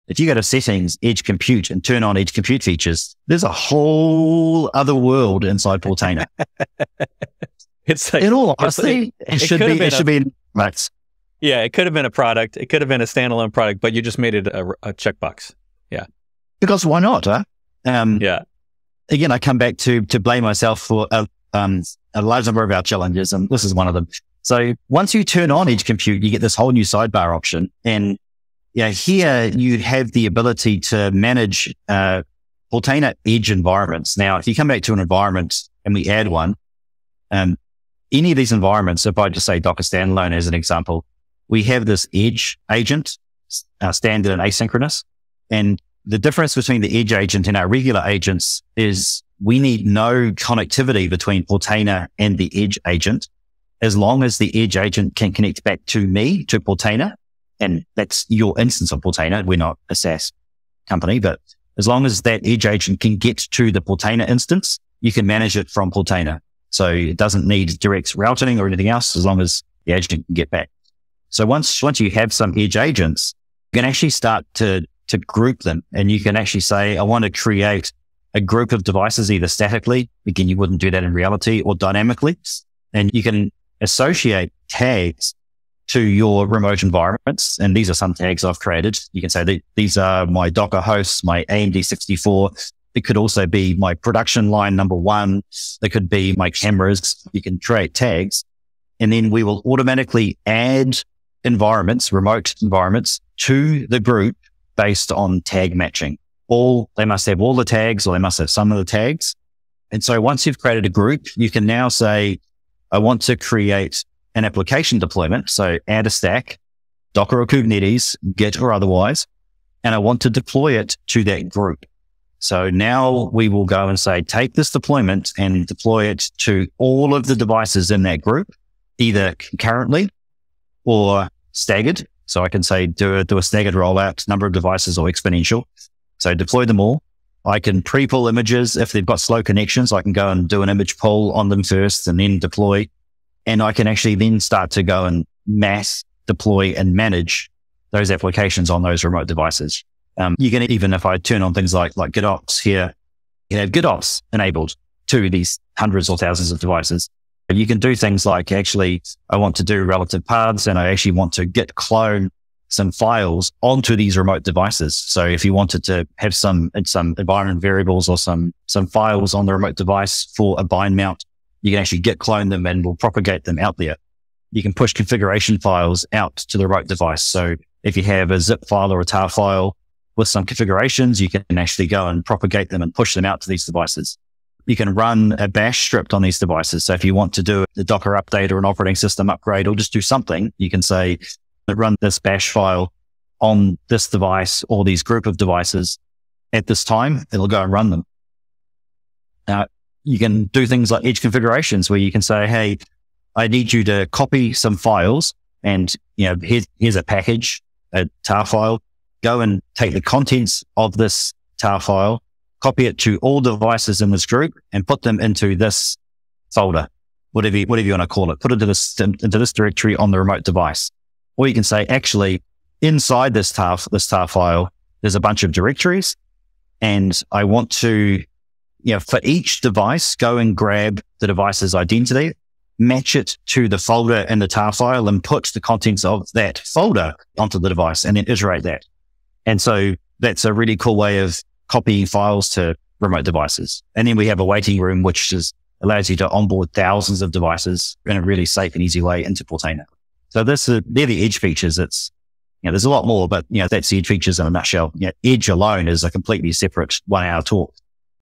25,000 Edge Nodes running Docker or Kubernetes inside Portainer - Live Show Clips
Portainer CEO Neil Cresswell showing how you could use Portainer to control up to 25,000 edge nodes of Docker or Kubernetes.

🗞️ Sign up for my weekly newsletter for the latest on upcoming guests and what I'm releasing

Bret Fisher